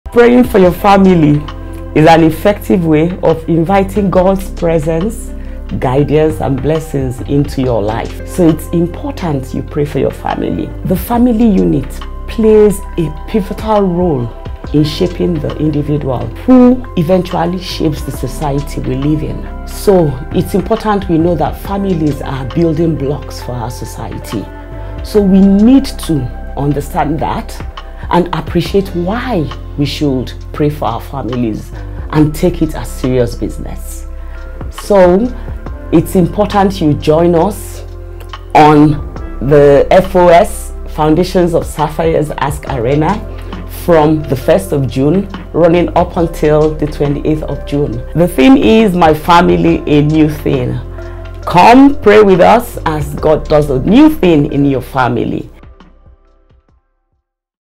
Praying for the family
Praying for the family is an effective way of inviting God's guidance and blessing into your life.